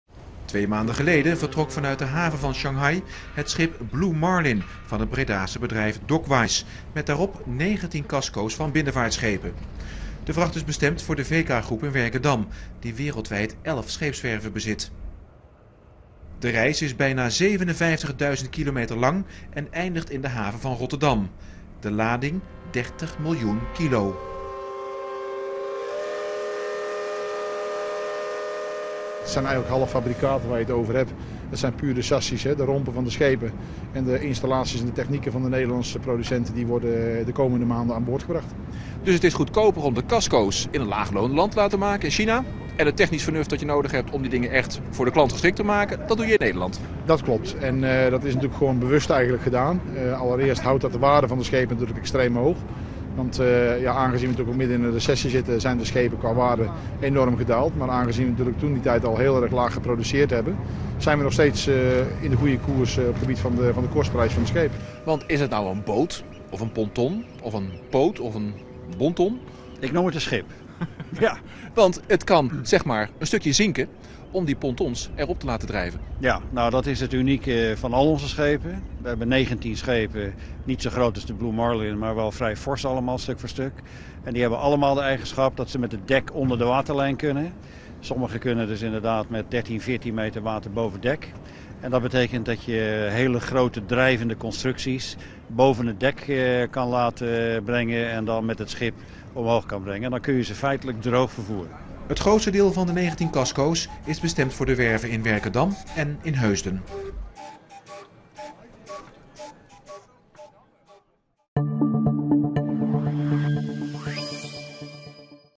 Feestelijk ontvangst in Rotterdamse haven
Een enorm transportschip van rederij Dockwise uit Breda kwam dinsdagmiddag aan in de Rotterdamse haven. Op het dek lagen negentien gestapelde binnenvaartschepen uit China. De scheepscasco's zijn bestemd voor de VEKA-werven in Werkendam en Heusden.